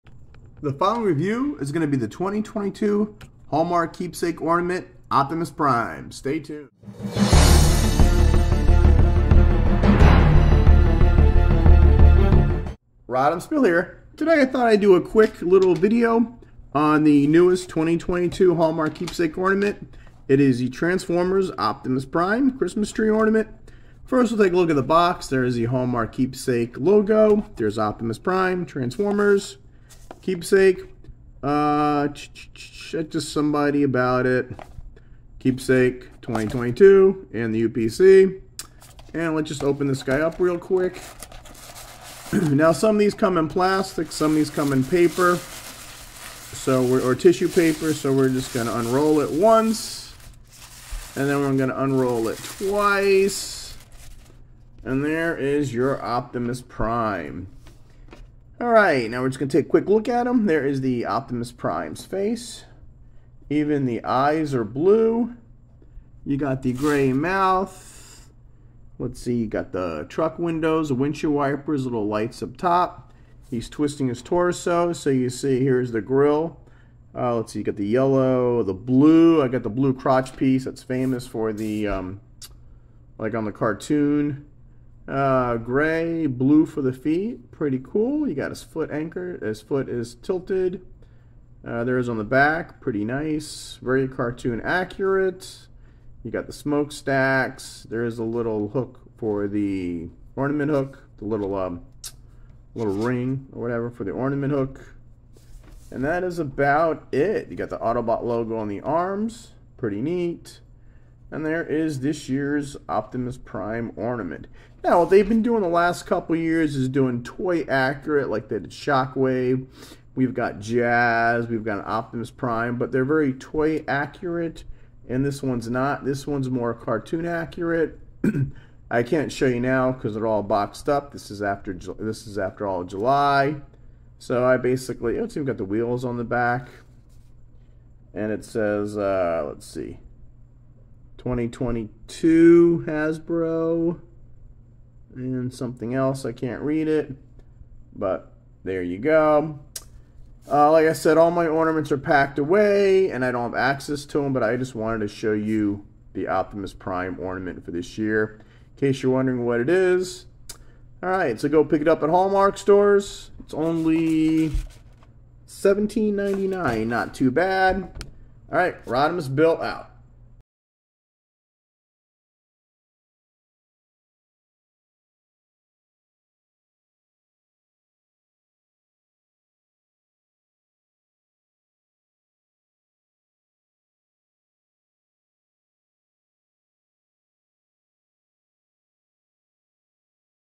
Transformers Optimus Prime Hallmark Keepsake Ornament 2022 - Rodimusbill Review
2022 Hallmark Keepsake Christmas Ornament Transformers Optimus Prime.